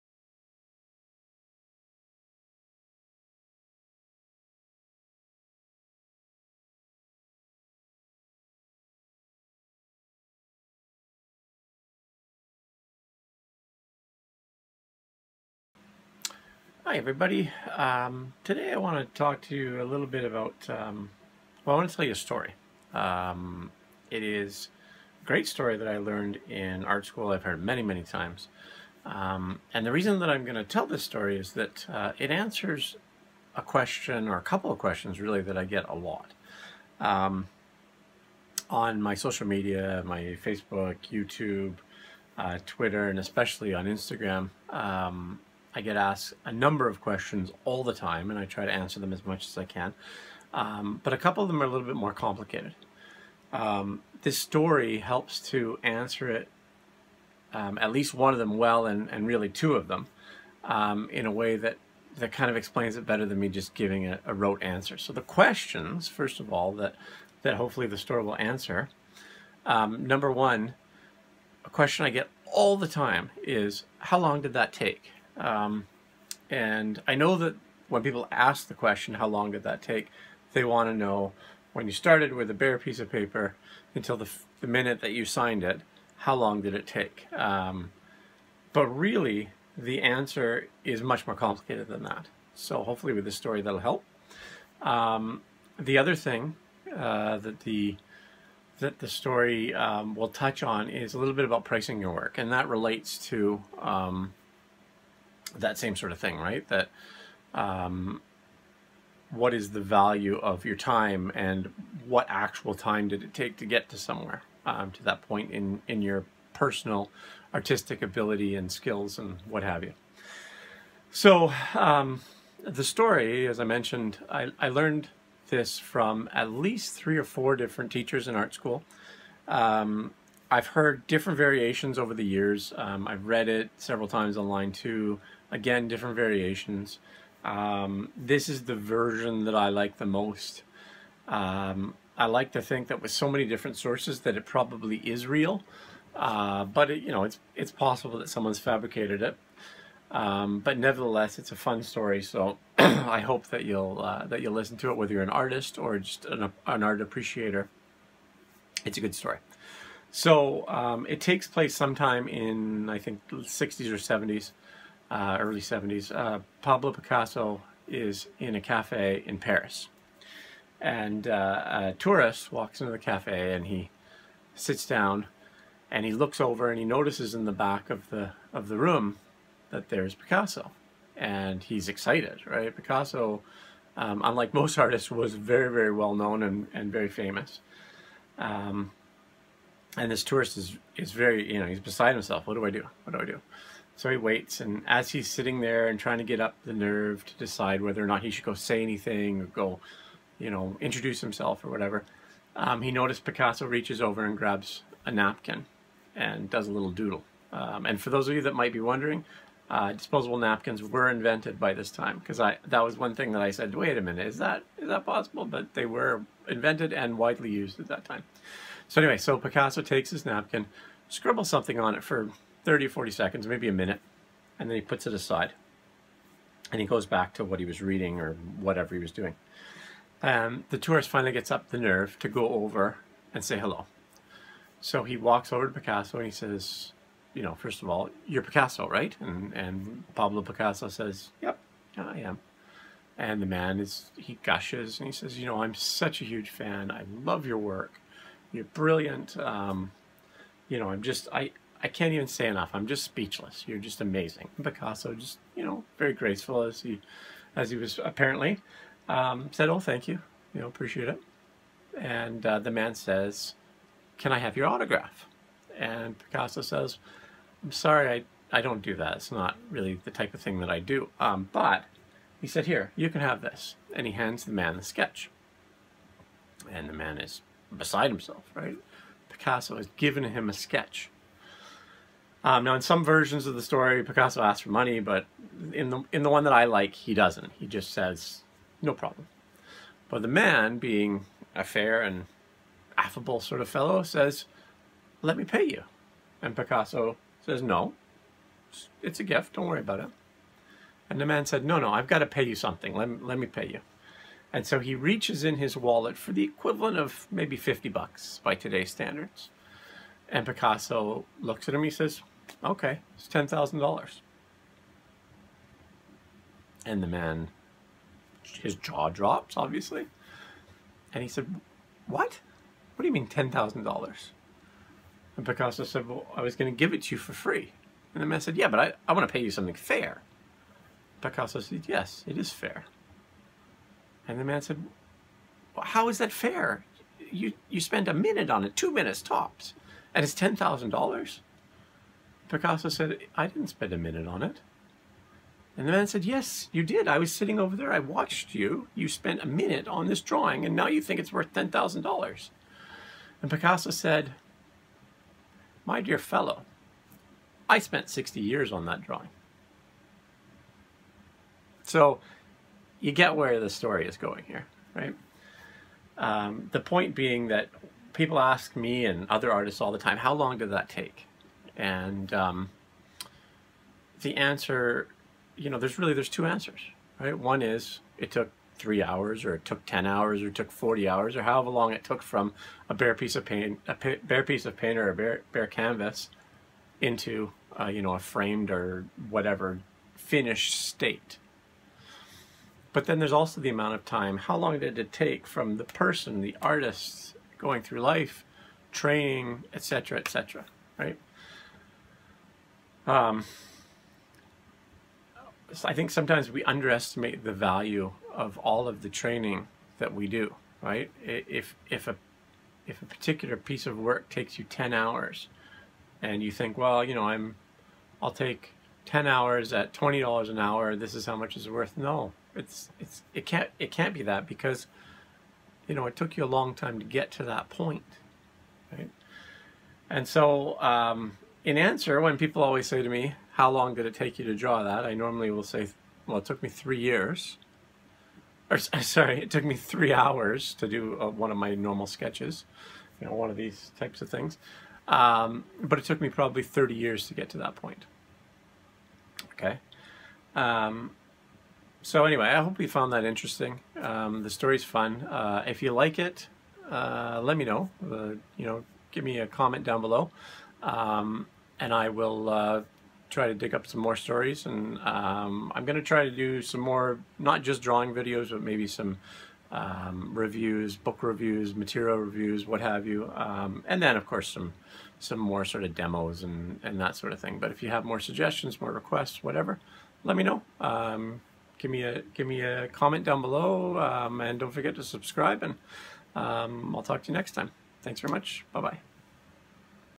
Storytime! Picasso, Pricing your Work - and - "How long did that take??"
Thought id try something a little different today - here's a video where i share one of my favourite stories. It helps explain why I answer the question "how long did that take" the way I do. I hope you find it interesting & useful!